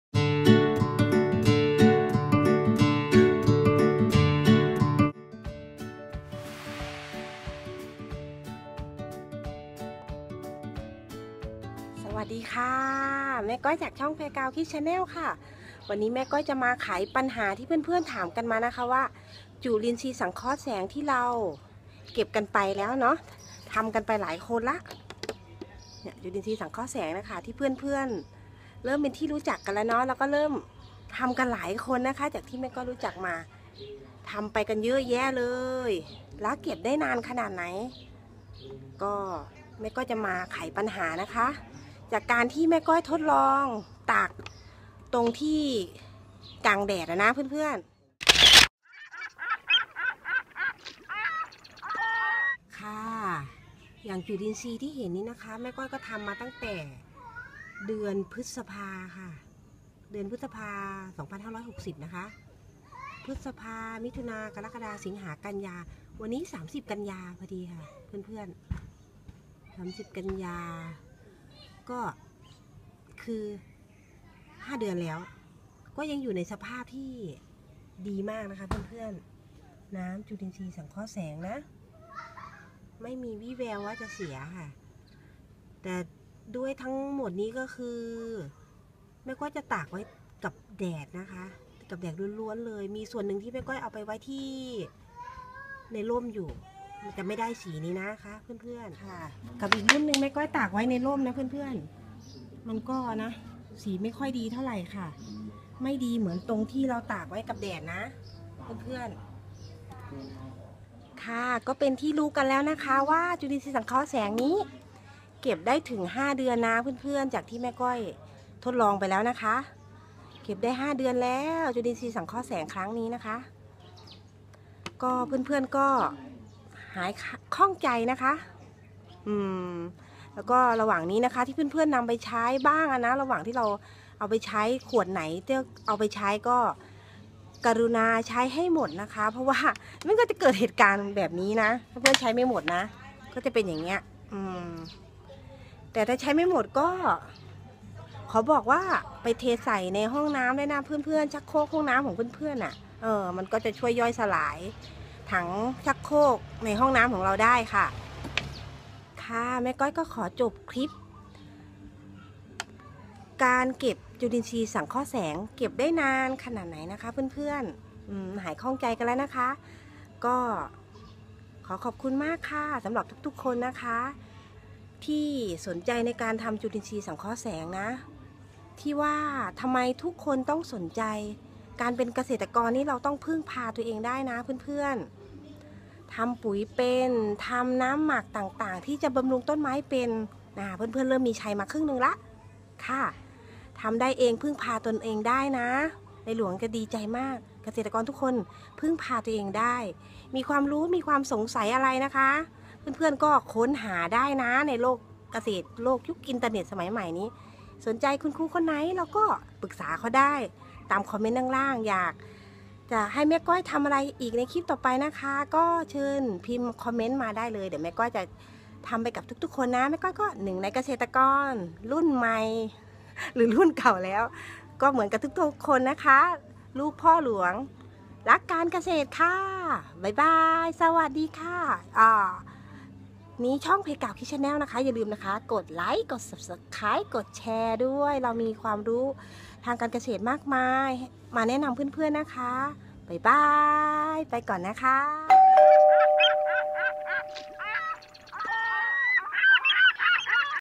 จุลินทรีย์สังเคราะห์แสงเก็บได้นานแค่ไหน แม่ก้อยพาทำ
น้ำจุลินทรีย์สังเคราะห์แสงที่ทุกคน ทำกันนั้นมันสามารถเก็บได้นานขนาดไหน มาทดลองพิสูจน์กัน ระหว่างการตากแดด กับการวางในร่ม ให้ผลเช่นไร สุดท้ายถ้ามีประโยชน์อย่าลืมกดติดตาม กดไมค์กดแชร์ ให้พวกเราด้วย ช่องplayground Kids chanel และเพจเดียวกัน